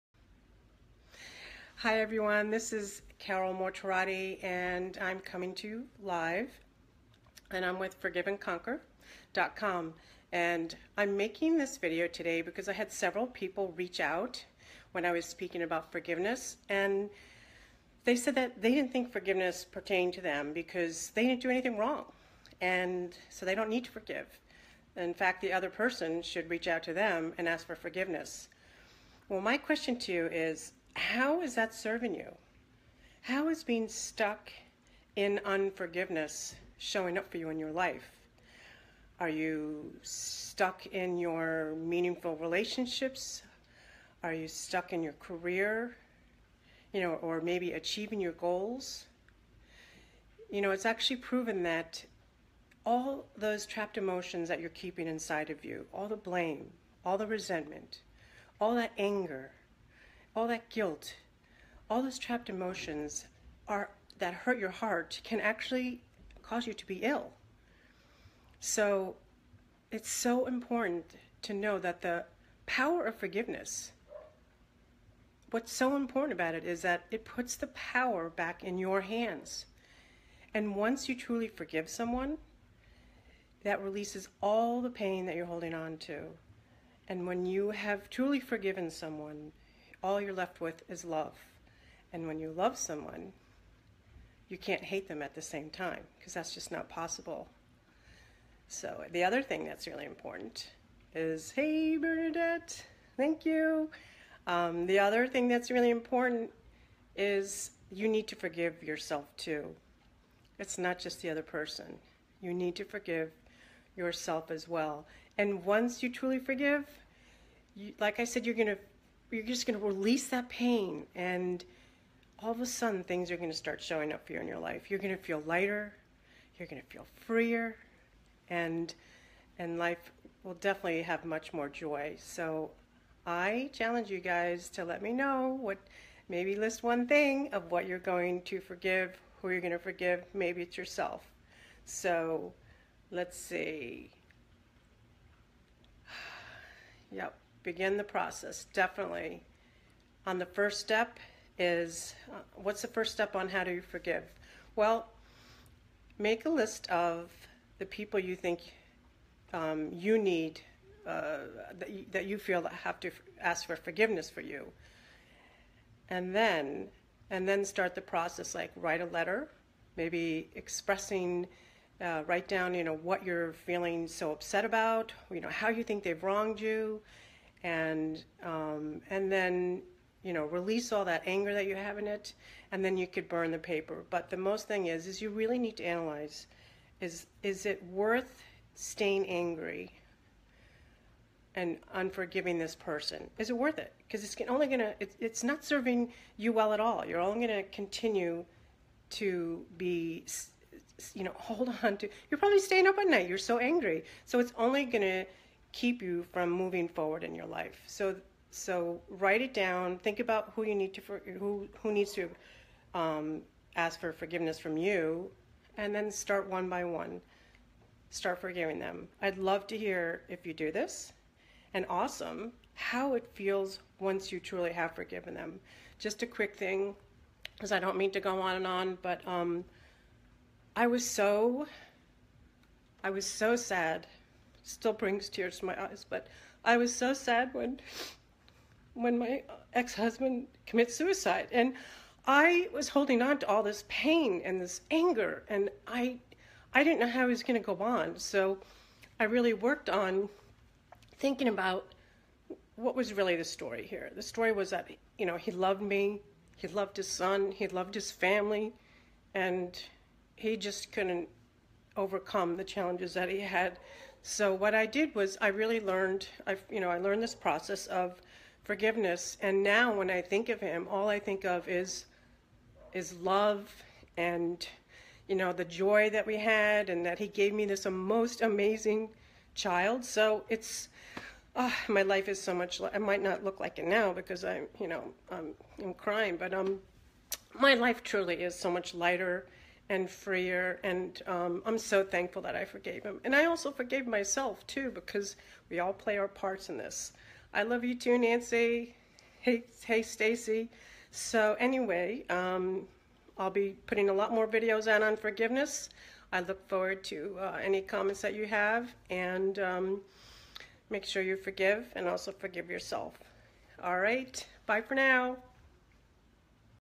The Power of Forgiveness - Grief Coach Carol Mortarotti
The Power of Forgiveness puts the power back in your hands and sets You Free! I know that your heart aches and you have been hurt from the past. You might have resentment or you’re holding on to anger or you have a loss from the past. You inspire me because I want to help you release the pain you’ve been holding on to and open up your heart to healing so you can be set free. Forgiveness is powerful. It puts the power fully in our hands and what that does is create a space for a brand new future that’s full of possibilities. You’re not alone. Through my experience of losing a loved one to suicide I am now able to share with you my wisdom and I am going to share with you how you can choose to release the pain, move through, and discover the power of forgiveness. Isn't it time you got your life back? Let me help you.